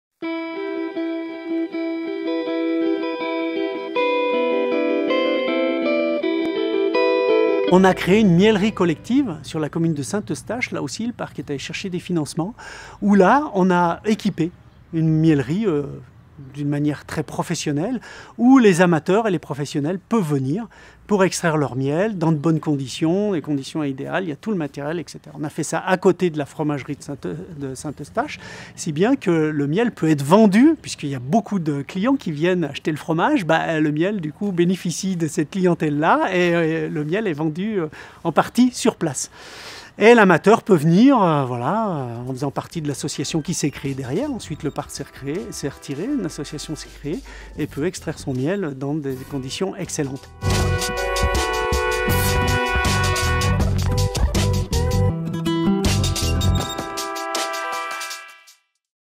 LE PNR des Bauges ouvre une miellerie coopérative pour soutenir les apiculteurs (VU)
Le PNR des Bauges a décidé de soutenir les apiculteurs en créant une miellerie coopérative.
À retrouver sur onpassealacte.fr